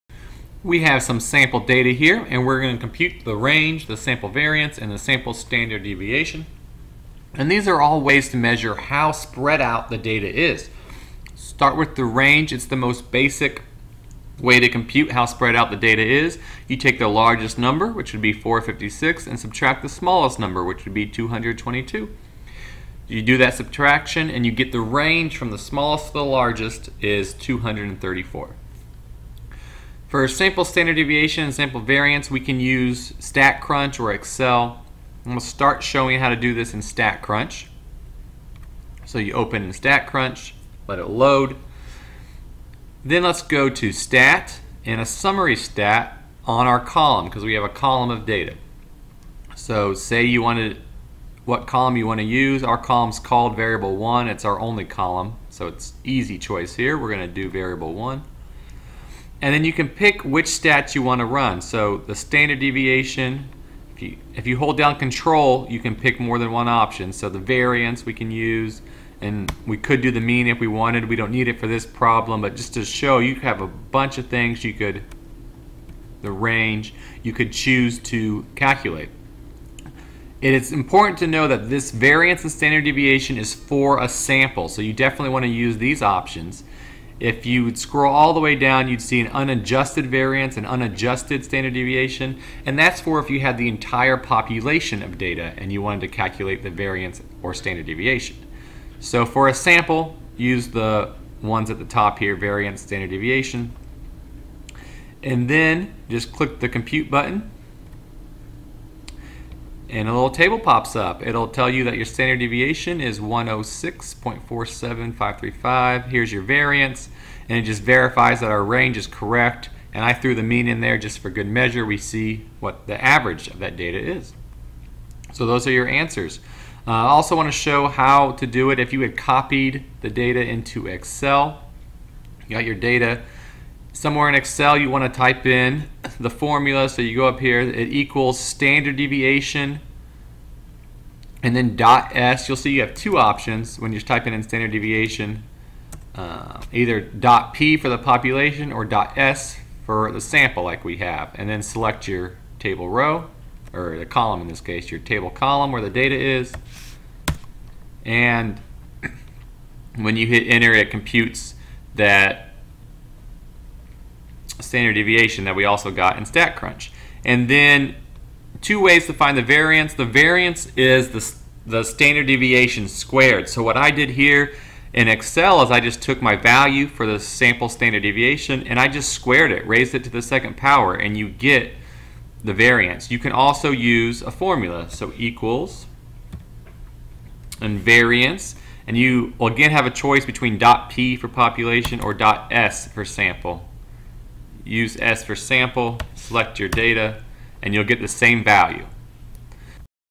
Sample: Range, Standard Deviation, Variance
How to find the sample Range, Sample Variance, and Sample Standard Deviation in both StatCrunch and Excel.